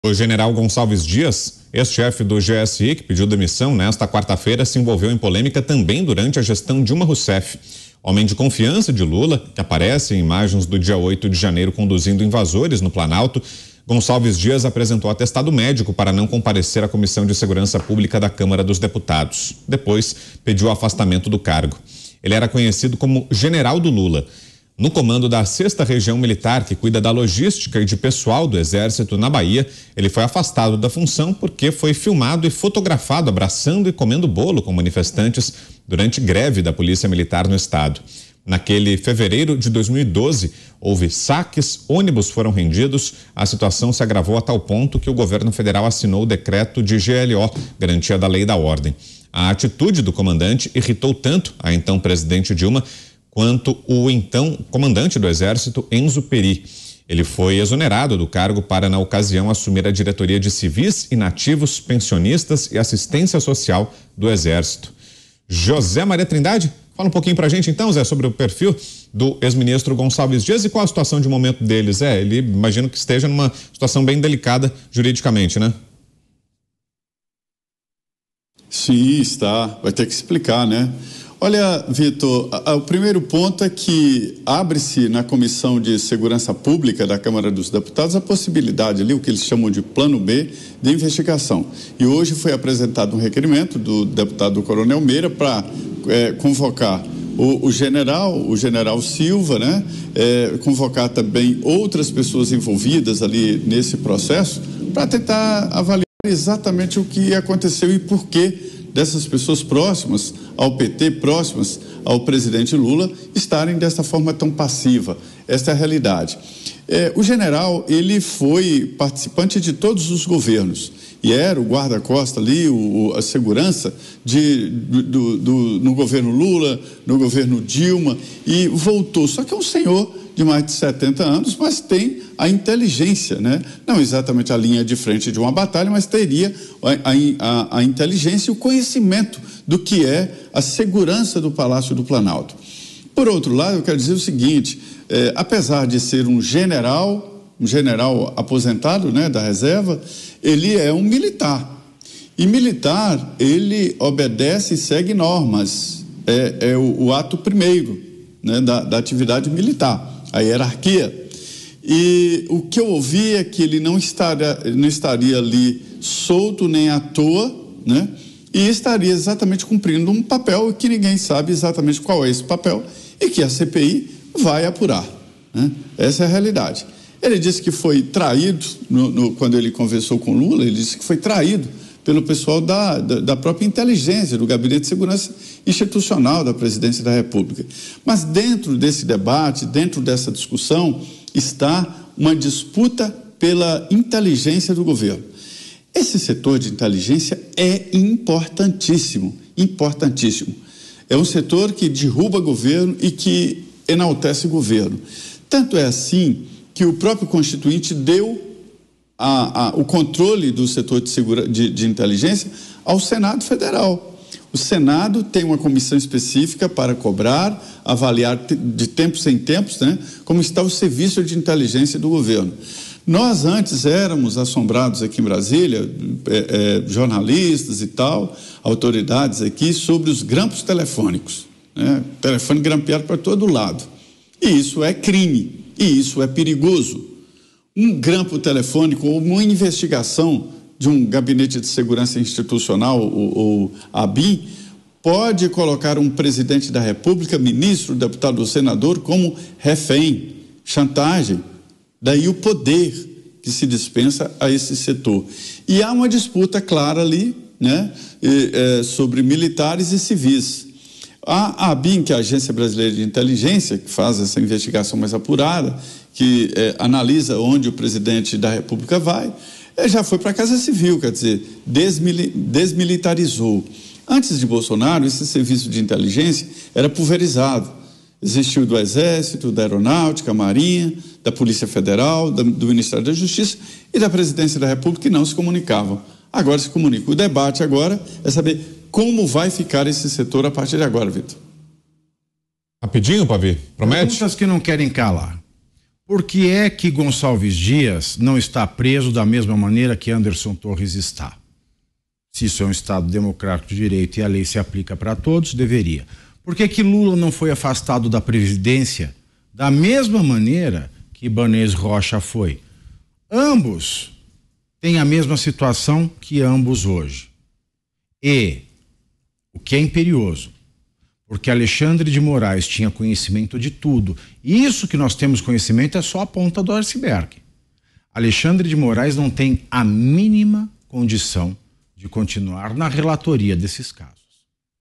General de Lula já havia sido punido na gestão Dilma
O ex-ministro-chefe do GSI, Gonçalves Dias, se envolveu em confusão também durante o governo Dilma. No comando da 6ª Região Militar que cuida da logística e de pessoal do exército na Bahia, ele foi afastado da função porque foi filmado abraçando e comendo bolo com manifestantes, durante greve da Polícia Militar no estado.